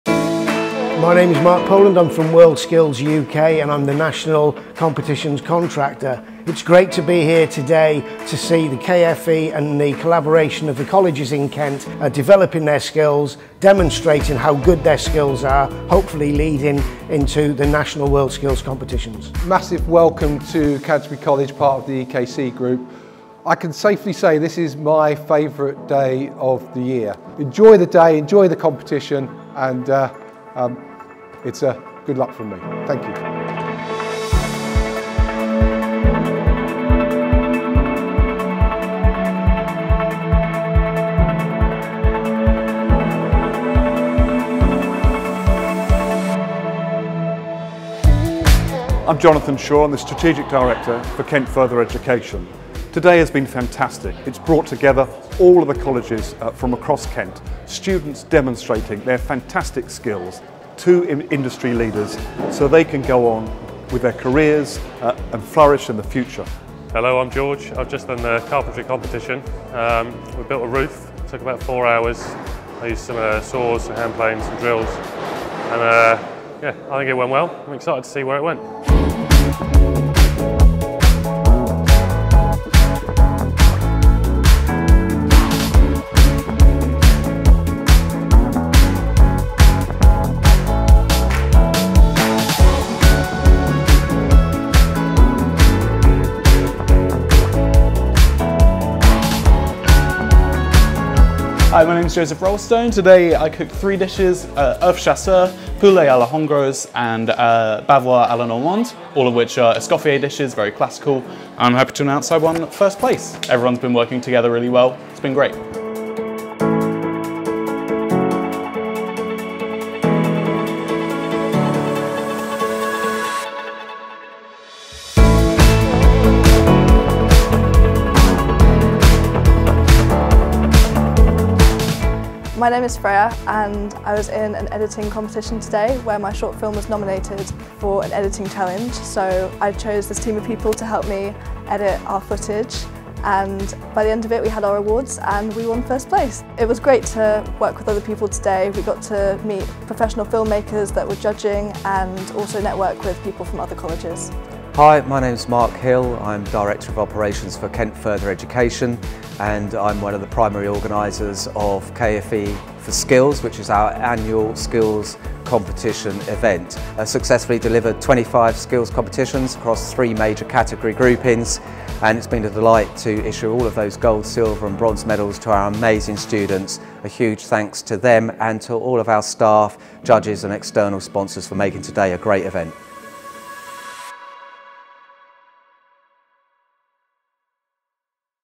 KMFM KFE4SKILLS EDIT LONG V5
This is the long form 3 minute video of KFE4Skills 2023 held at Canterbury College on 10th March 2023.